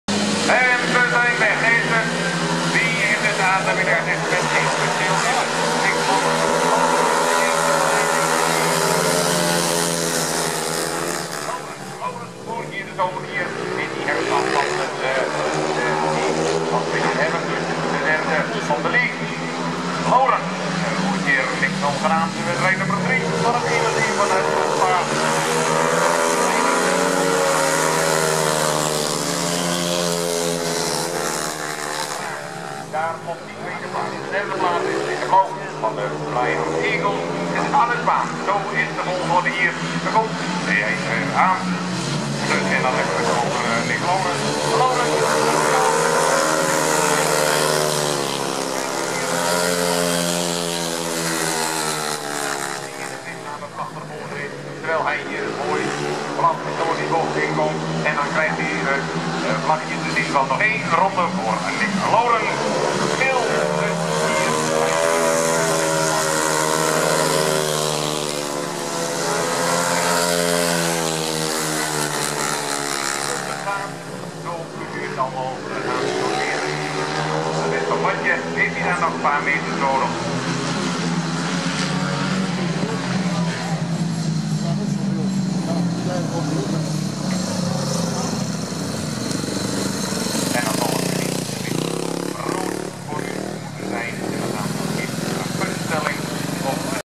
speedway veenoord 31 mei 2014 3e heat 1e nick lourens 2e henk koonstra 3e anne spaan
dutch speedway league veenoord 31 mei 2014. 3e heat. 1e nick lourens. 2e henk koonstra. 3e anne spaan. 4e ramon woortman.